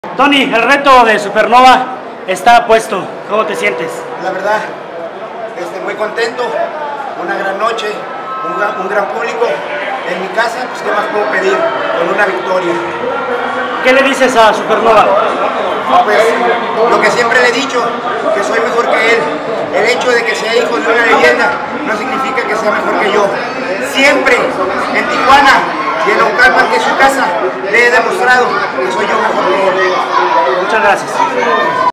A pesar de que eres hijo de una leyenda el mejor soy yo Súper Nova "TONY CASANOVA"
Tony Casanova crecido por su victoria ante Súper Nova y se siente mejor que el en Tijuana o en Naucalpan.